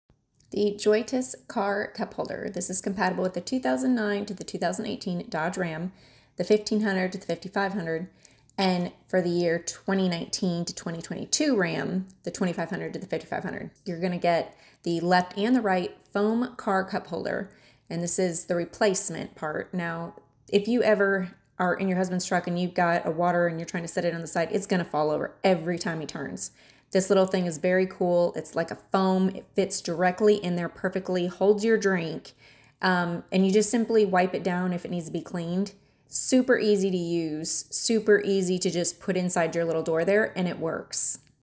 MUST HAVE Dodge Ram Accessories - Door Cup Holders
The Joytus Car Cup holder. This is compatible with the 2009 to the 2018 Dodge Ram, the 1500 to the 5500, and for the year 2019 to 2022 Ram,
the 2500 to the 5500. You're going to get the left and the right foam car cup holder, and this is the replacement part. Now, if you ever are in your husband's truck and you've got a water and you're trying to set it on the side,
it's going to fall over every time he turns. This little thing is very cool. It's like a foam. It fits directly in there perfectly. Holds your drink and you just simply wipe it down if it needs to be cleaned.
Super easy to use. Super easy to just put inside your little door there and it works.

You can find the MUST HAVE Dodge Ram Accessories- The Door cup holders on Amazon